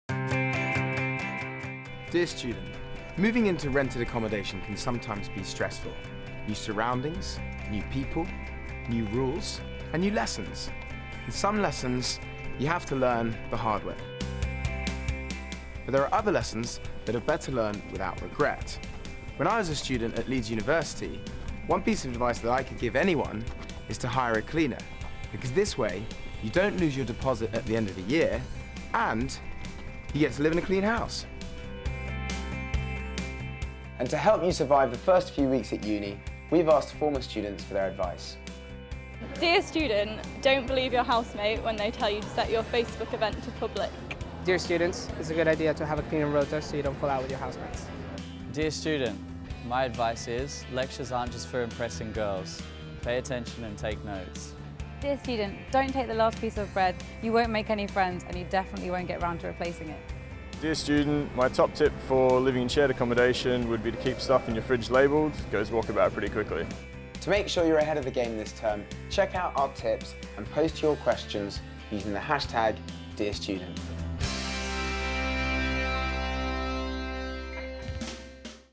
Dear Student
Former student and Made in Chelsea star Andy Jordan shares some tips on student living, dealing with landlord and letting agents, and how to get the most from university life. He also invites other former students to share some of their own tips.